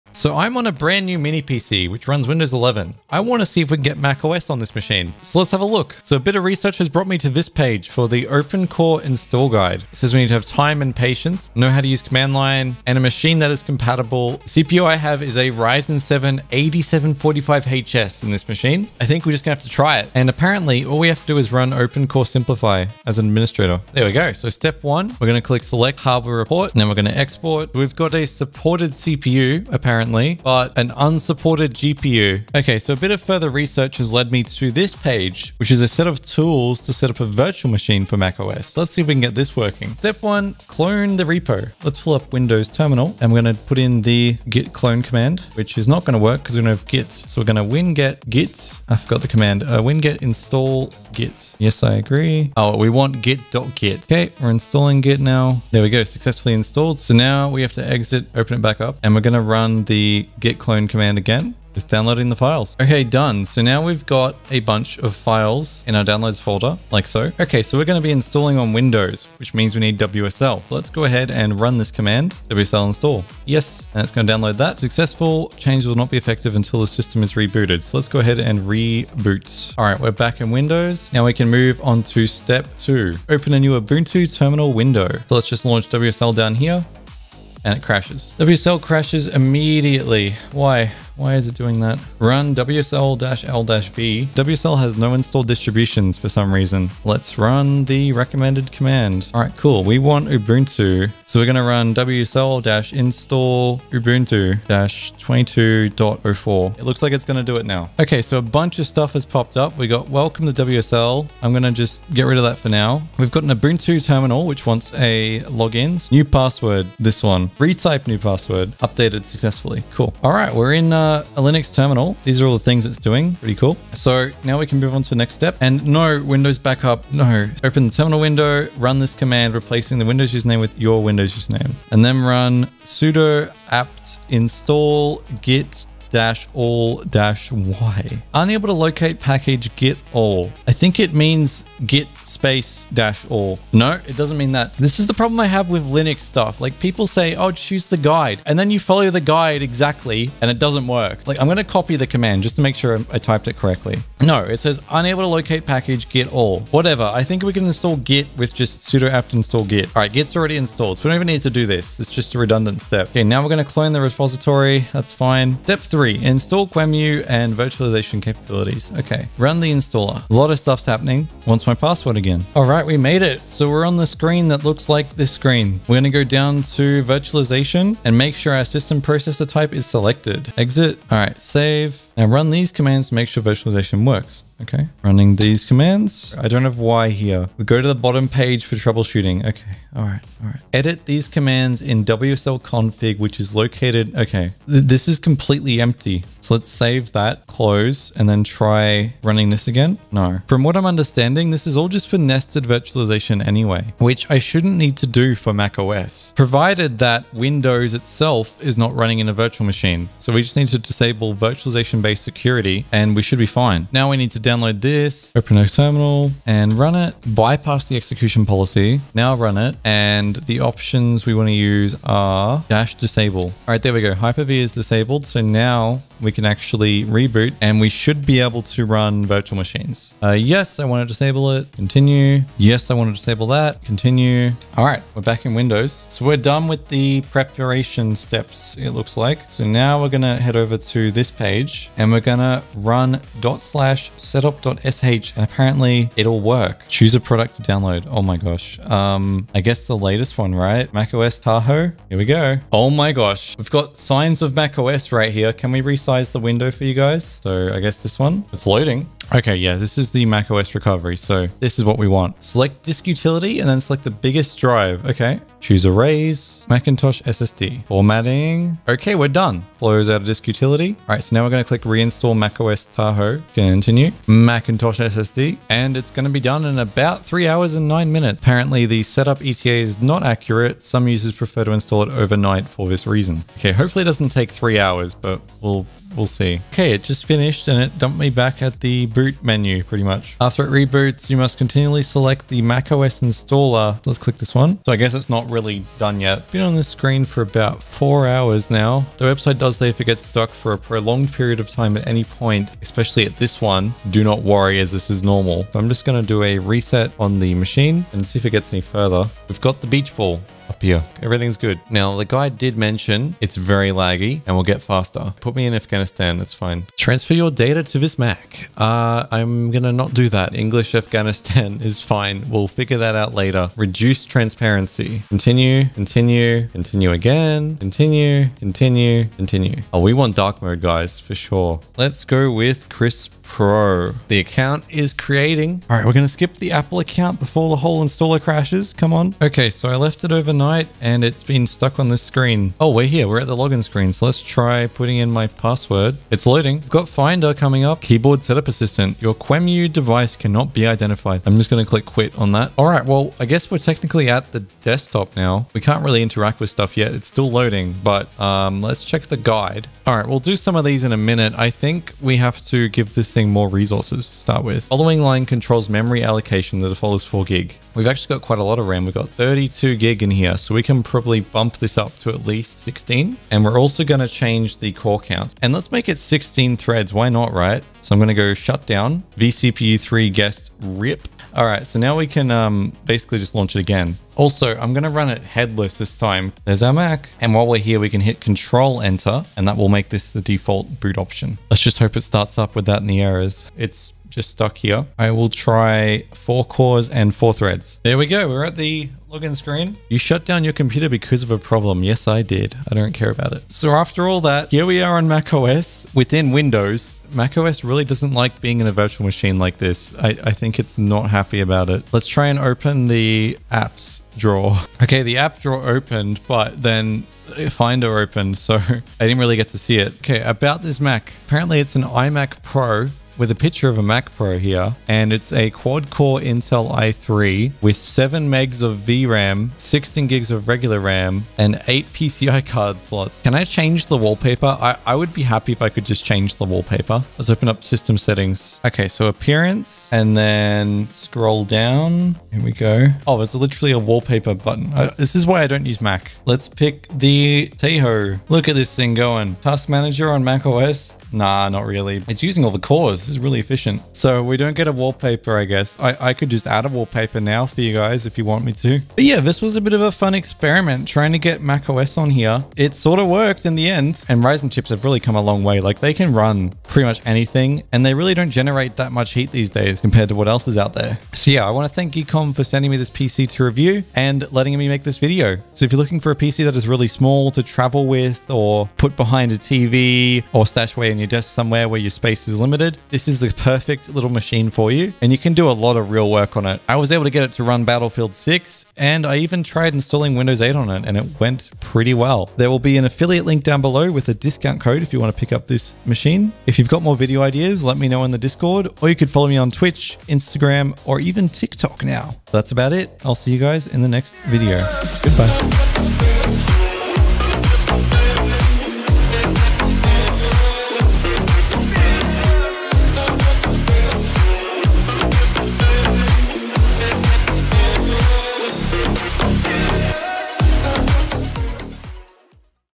Can a Modern PC Run MacOS?
In this video we try and get MacOS running on a Mini PC.

💸 Code: CPGKA812

💸 Code: CPA86OFF

💸 Code: CPA86OFF

💸 Code: CPGKA812

If you ever need help or want a place to chill consider joining my Discord!